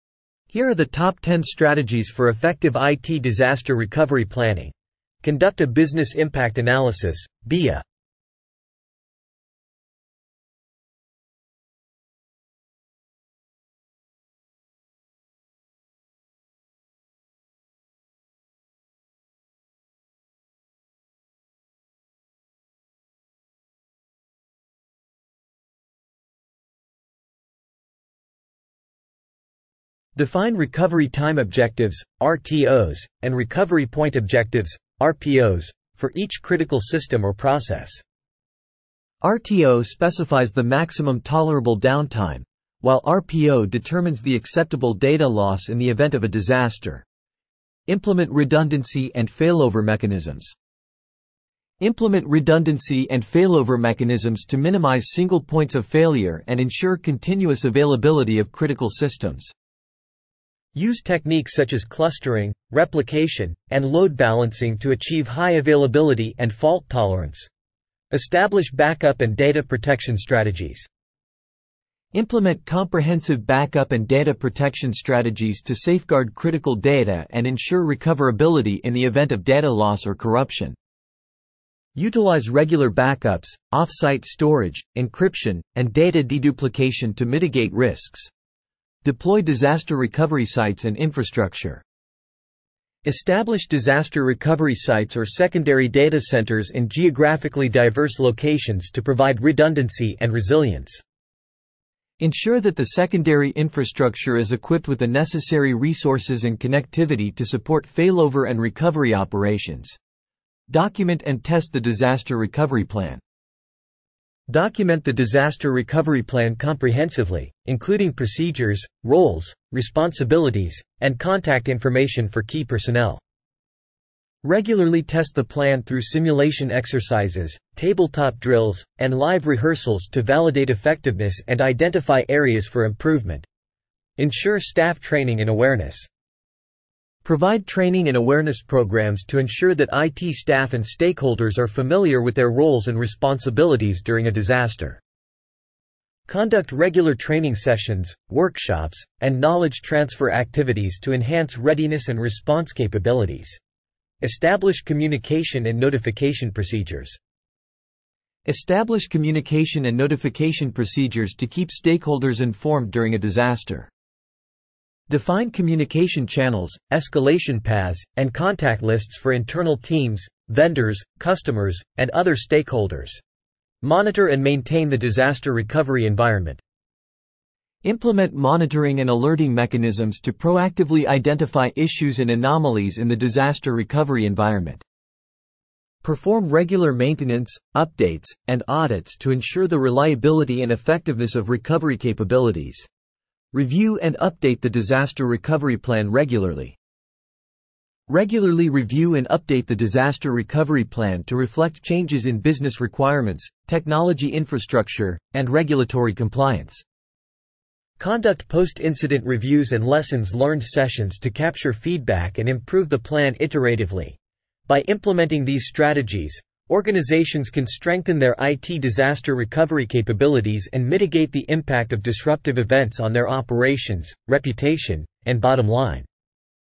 Top 10 Strategies for IT Disaster Recovery Planning
Prepare for the unexpected with our top 10 strategies for IT disaster recovery planning. Ensure your business continuity and data protection with these essential tips. From backup solutions to recovery processes, these strategies will help you safeguard your IT infrastructure.